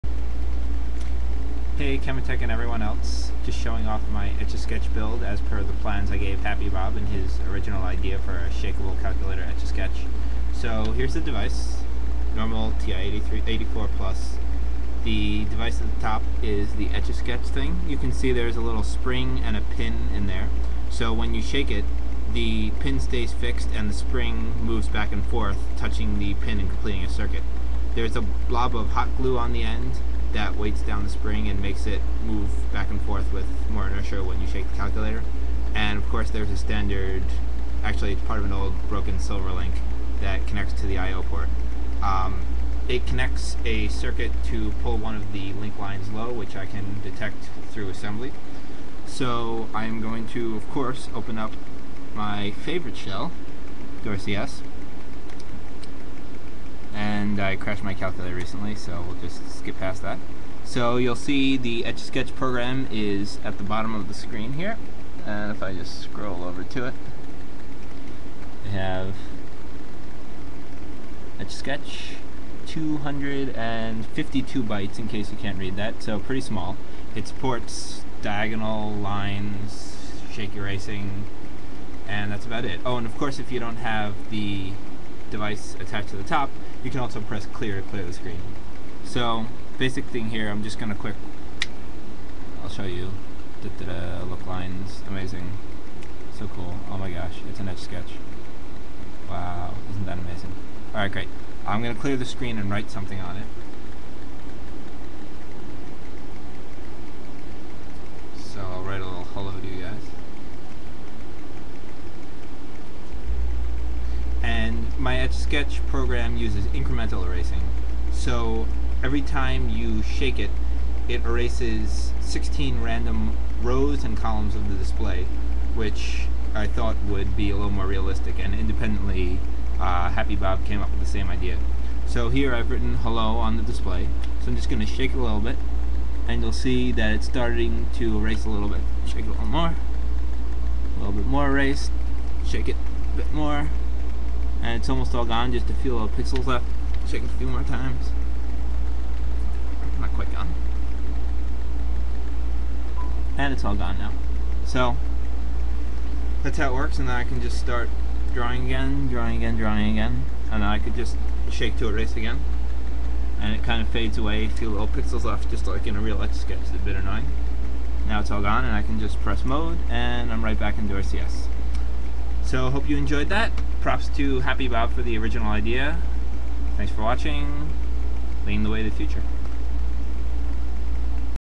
TI-84+ Etch-A-Sketch With Real Shaking
As per an idea by user HappyBob on Cemetech and executed in hardware and software by yours truly, a calculator Etch-A-Sketch that you can really shake.